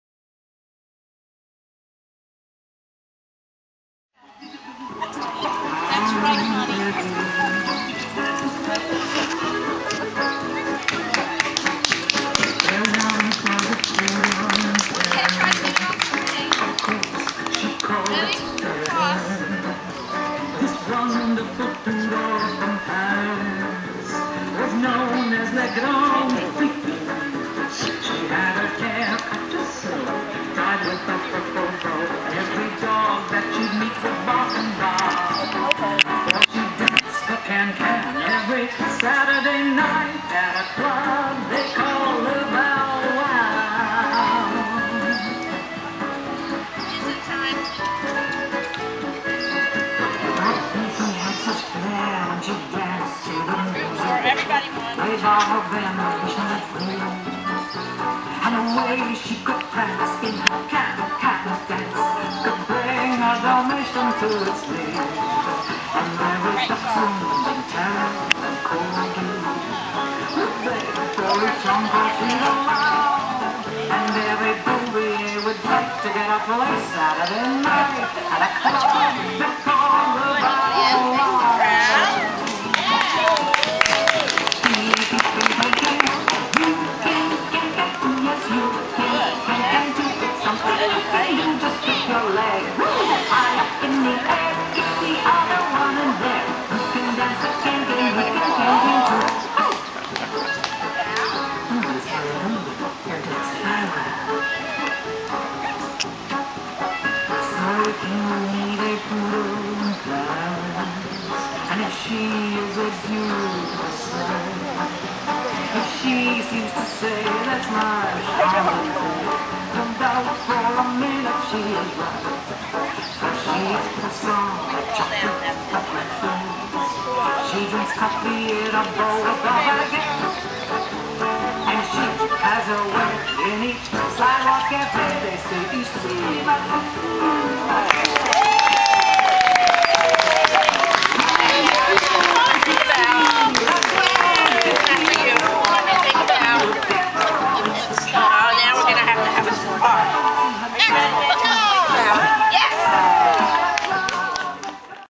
Dancing Poodles! "The Pom Poms" Musical Freestyle Team
Freestyle demo in Brimfield, Mass.  2012  We all really enjoy this event (performed last year and were invited back).